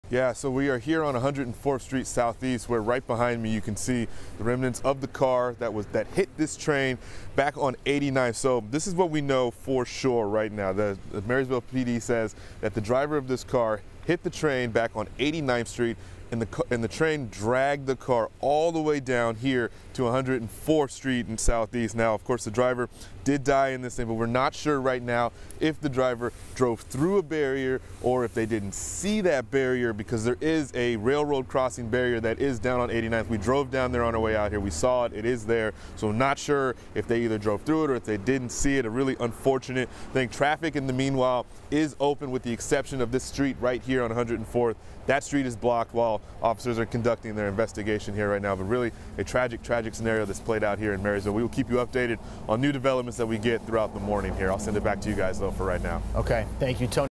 Marysville police respond to fatal accident
A car was hit and dragged by a train.  The driver was killed.